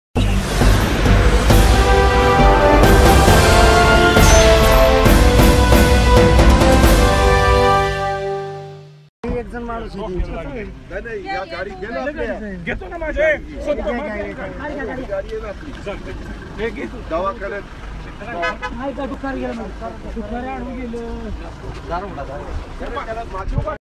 सातारा : अजित पवारांकडून अपघातग्रस्तास मदत
महाबळेश्वर-सातारा रस्त्यावर मंगळवारी रात्री ९ वाजता एक अपघात झाला. अपघातग्रस्त संतोष बजरंग जाधव (रा. बिबवी) यांना अजित पवार यांनी मदत करून पुन्हा एकदा माणुसकीचे दर्शन दिले.